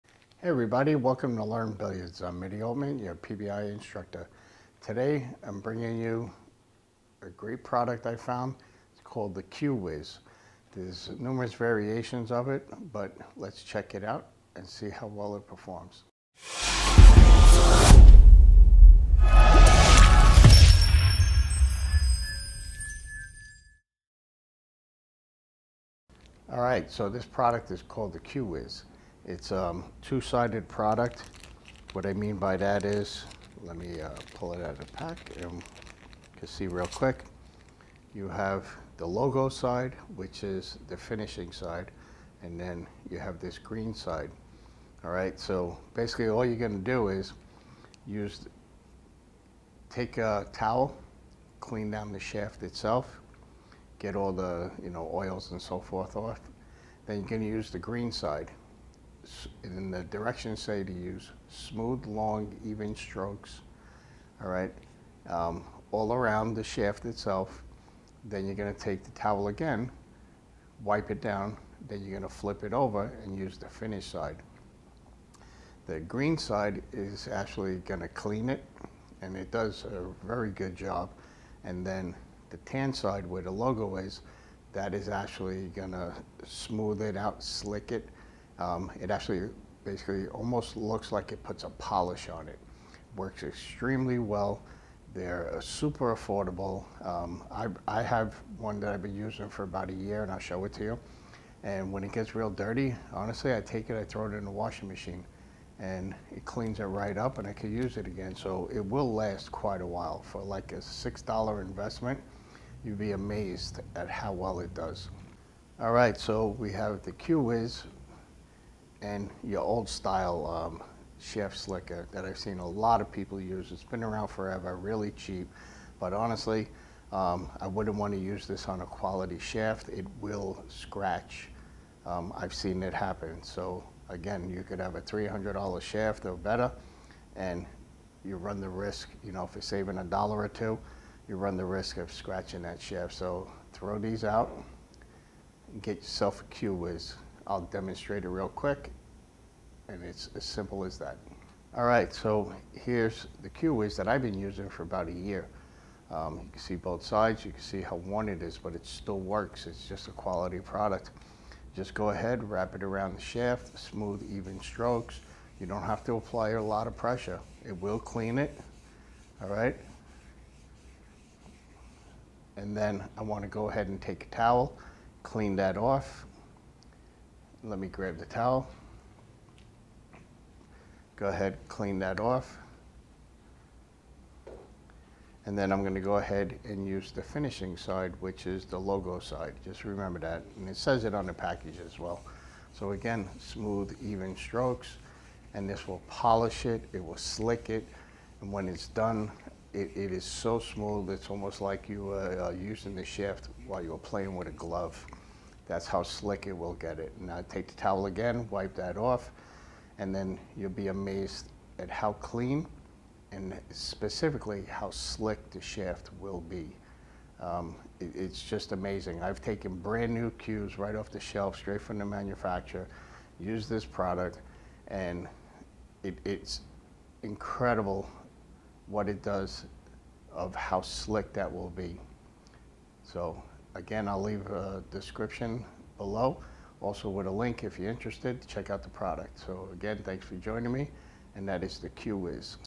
Pool Cue Shaft Maintenance
Pool Cue Shaft Maintenance, this Q Wiz pool cue shaft maintenance product will leave your pool cue shaft like new and super super slick.  Make you pool cue shaft better then new with the Q Wiz shaft cleaner/conditioner